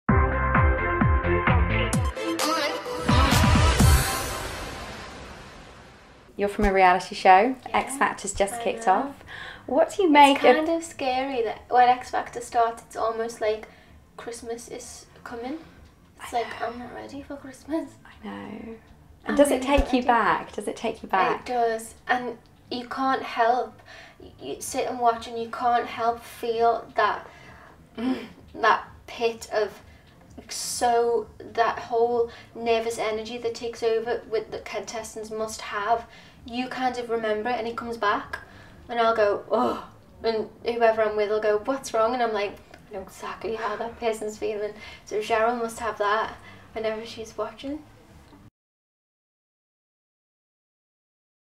Nicola Roberts On The X Factor!
Girls Aloud's Nicola Roberts says the nervous energy she felt when auditionning on a TV talent show comes back when she watches X Factor.